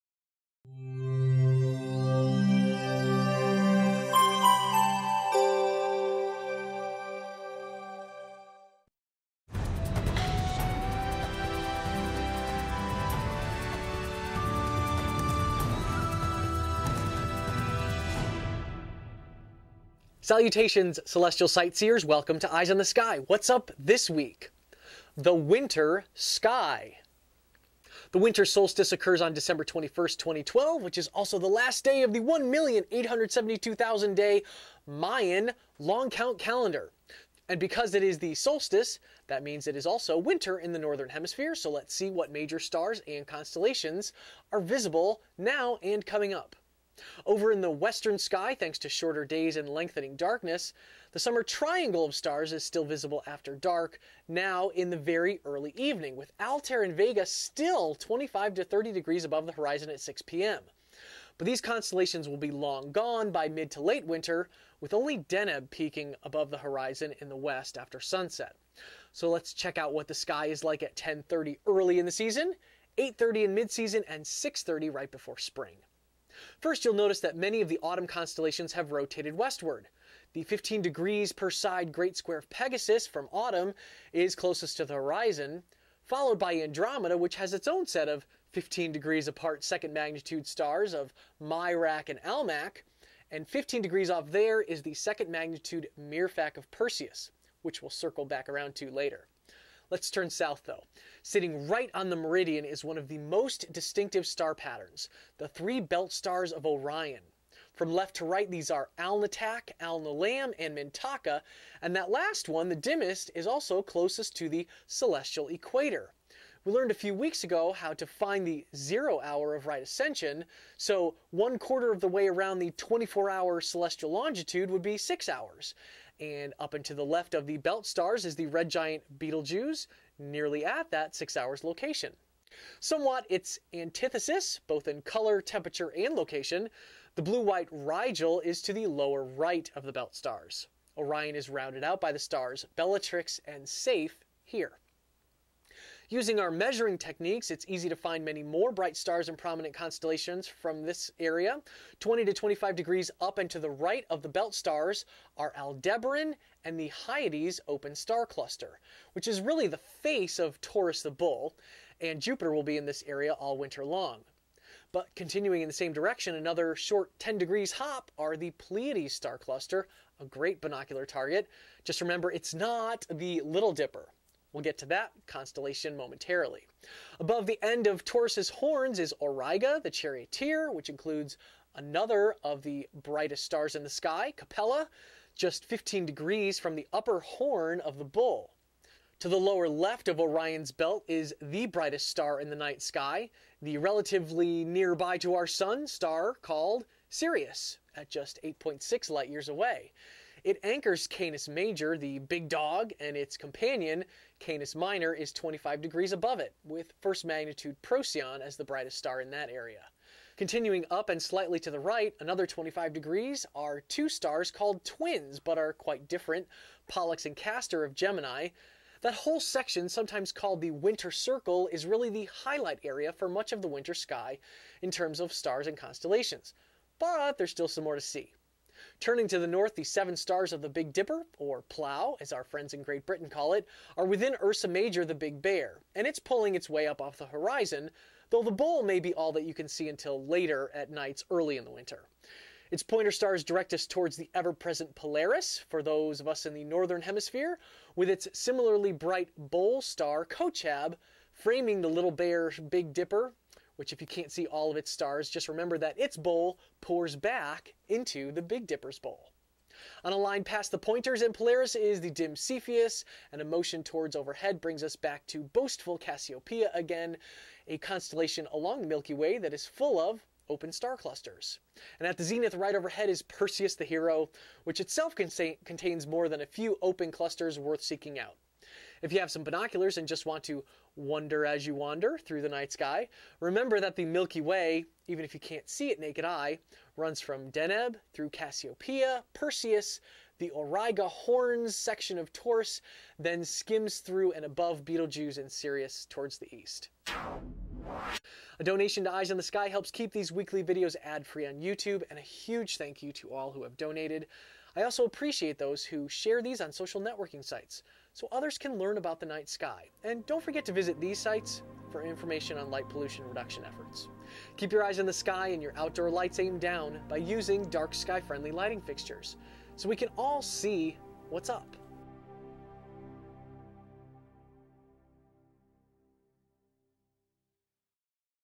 Eyes on the Sky: Dec 17 thru Dec 23
The winter solstice provides the prelude to the prominent winter patterns in the sky.